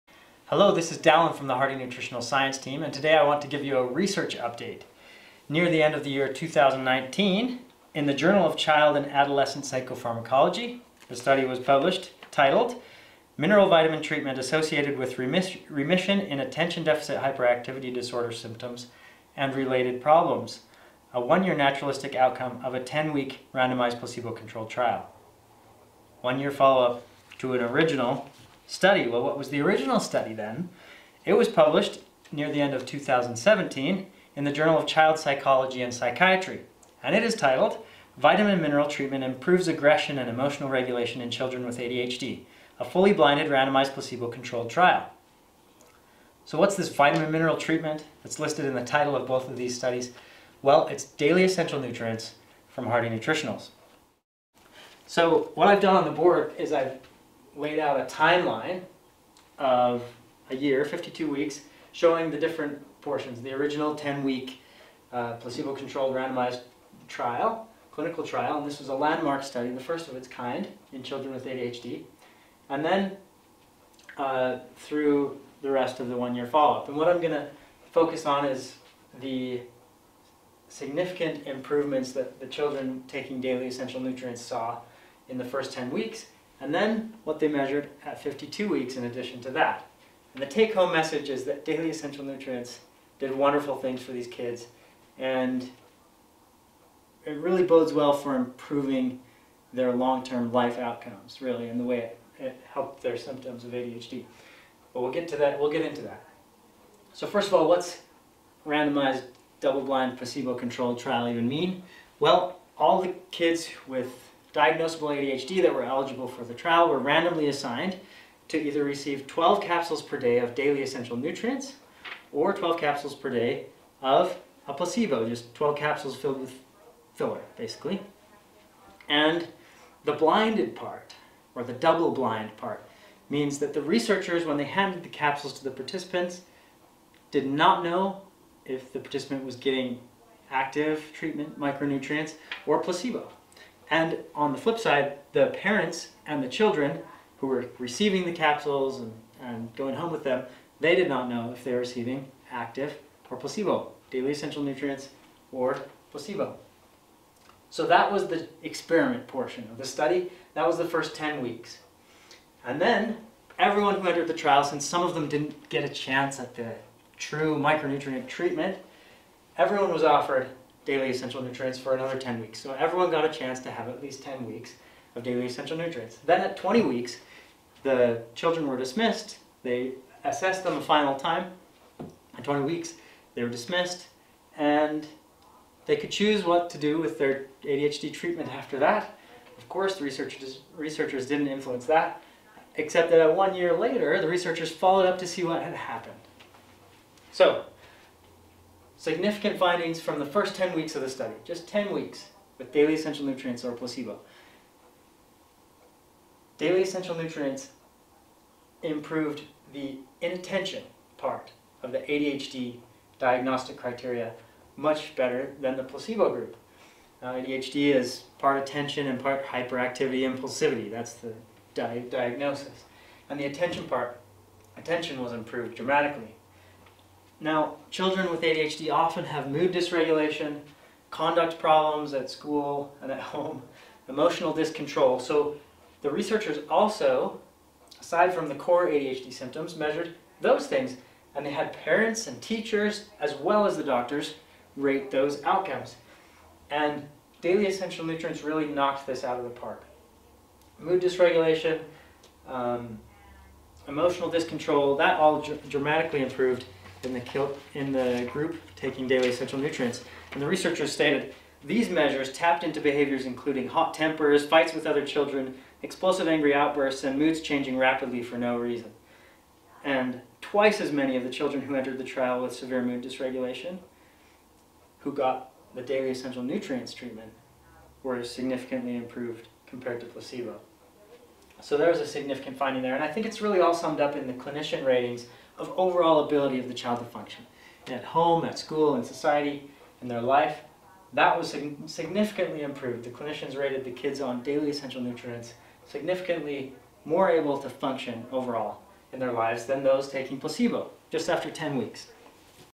Micronutrients New Mental Health Research Part 1
Dallin from the Hardy Nutritionals® science team shares an ADHD research update on Hardy's Daily Essential Nutrients, a clinical micronutrient therapy designed to alleviate mood and mental health symptoms.

Daily Essential Nutrients is backed by multiple independent studies for mood and mental health, including recent double-blind research on ADHD.
In 2019, a study titled "Mineral-Vitamin Treatment Associated with Remission in Attention-Deficit/Hyperactivity Disorder Symptoms and Related Problems: 1-Year Naturalitic Outcomes of a 10-Week Randomized Placebo-Controlled Trial" was published in The Journal of Child and Adolescent Psychopharmacology.  This study was a follow-up to the original study which was published in The Journal of Child Psychology and Psychiatry in 2017 titled "Vitamin-mineral treatment improves aggression and emotional regulation in children with ADHD: a fully blinded, randomized, placebo-contnrolled trial".

The vitamin-mineral treatment studied is Daily Essential Nutrients, formulated by Hardy Nutritionals®.

The original trial is a landmark study for ADHD, the first of its kind.  The research demonstrated significant improvements in the children's symptoms in both the original study and the one-year follow up trial.  While Daily Essential Nutrients is clinically validated for use across a wide range of mood and mental health disorders for both children and adults, this new research focuses specifically on children with ADHD.

So first of all, what is a Randomized Double-Blind Placebo-Controlled Trial?

A RDBPCT is "a medical study involving human participants in which neither side knows who's getting what treatment and placebo are given to a control group".  Therefore, participants were given either the Daily Essential Nutrients or placebo, without knowing which they were receiving.  In addition, the researchers also were "blinded" to which capsules the study participants were getting.  However, those who did not receive the micronutrients during the initial 10-week trial period, were able to try the micronutrients for 10-weeks after their placebo trial.  At the conclusion of the 20-week period, researchers made a final assessment of each participant and took a look at the data to make some conclusions.  One year later, the researchers followed-up again to see what the outcomes were long-term.

What were the significant improvements seen from using Daily Essential Nutrients?
 Daily Essential Nutrients improved the "inattention" part of ADHD much better than placebo in the study.  Children with ADHD often have mood dysregulation problems and researchers measured those outcomes as well, rated by both parents and teachers.  Daily Essential Nutrients dramatically improved both inattention/impulsivity and mood control in the children with ADHD.  "These measures tapped into behaviors such as hot tempers, fights with other children, explosive angry outbursts, moods changing rapidly for no reason."

Twice as many children who received the Daily Essential Nutrients were significantly improved compared to those who received placebo.  The researchers found that the children studied were better able to function overall just after 10 weeks of taking the Daily Essential Nutrients micronutrient blend.